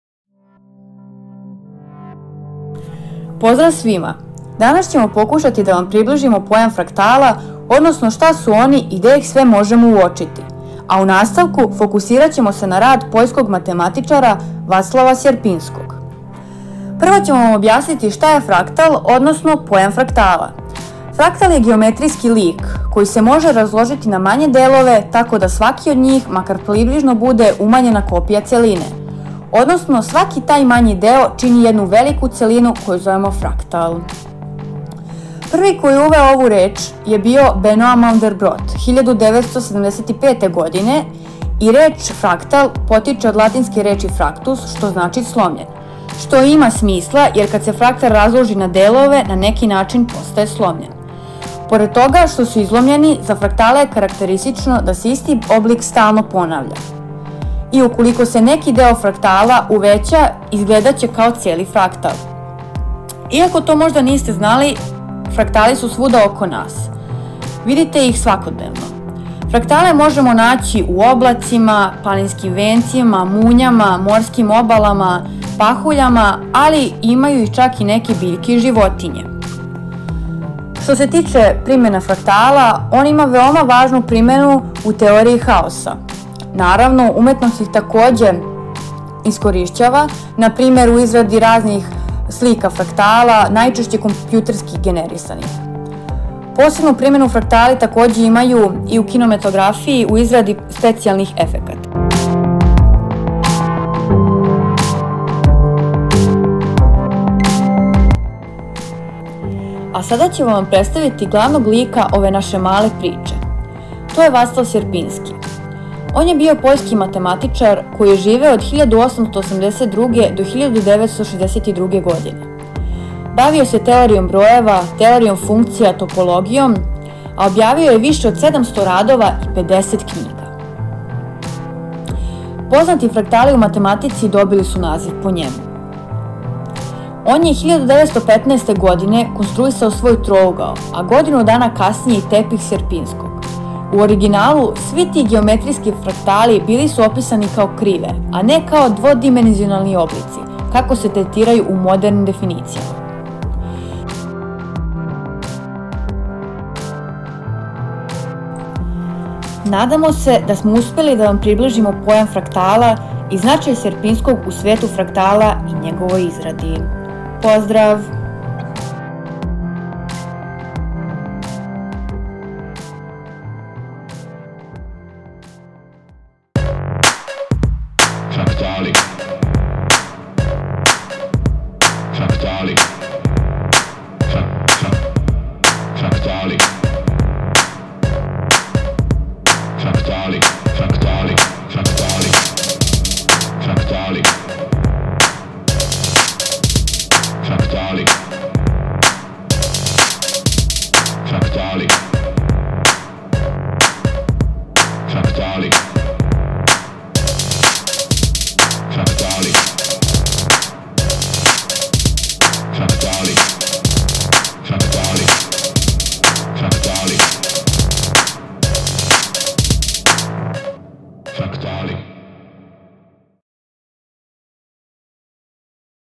Фрактали
Kratak film o fraktalima Ivan Zafirović, Relja Peršić, Jelica Radović, Oleg Svoboda, Nikolina Karkić, Una Mihailović, Predmet-Umetnost i dizajn, nastavnica muzicke kulture Snezana Filipovic, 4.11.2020, Prva beogradska gimnazija